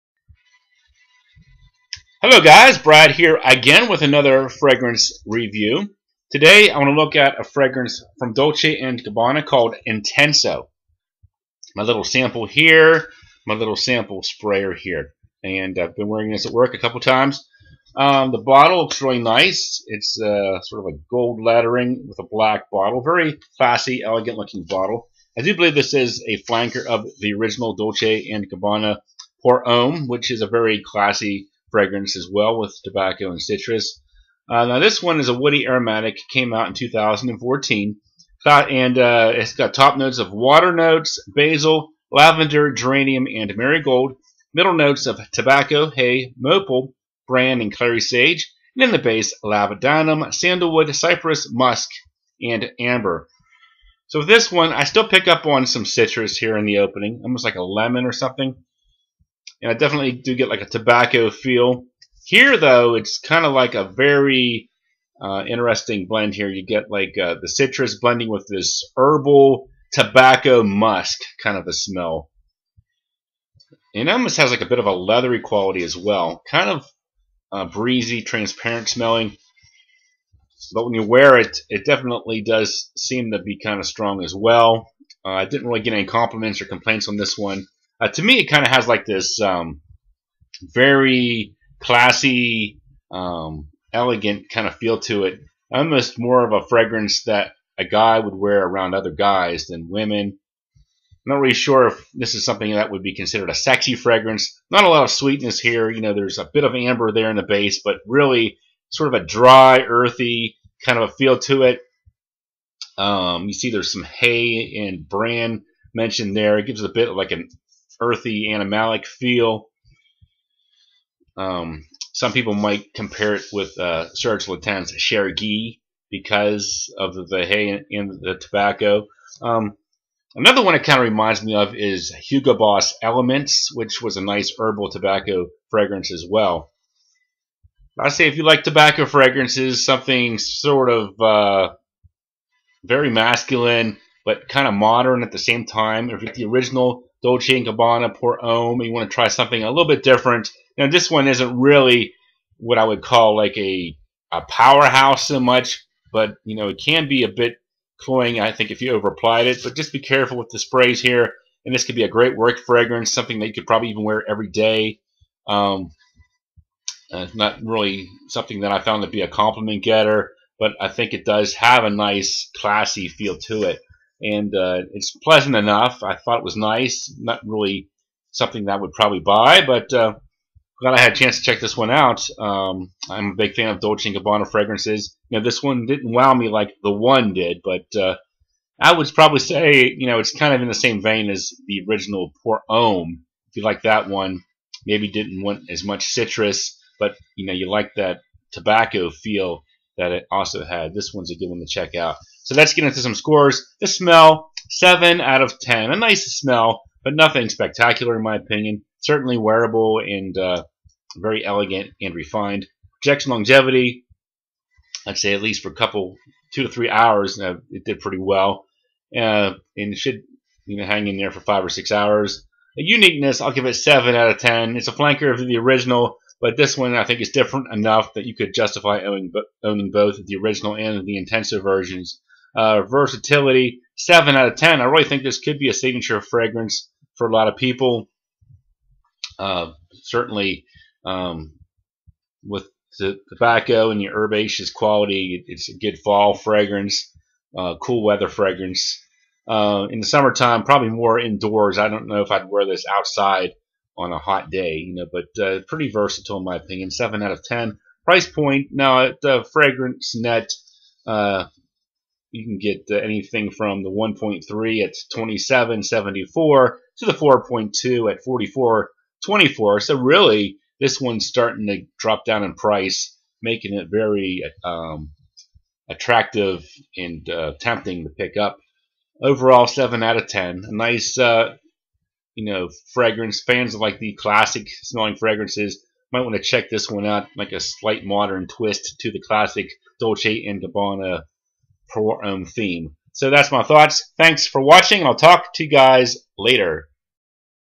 Dolce & Gabbana Intenso Review
A flanker of the classic Pour Homme. Less citrusy than the original. An herbal tobacco musk fragrance with some earthy tones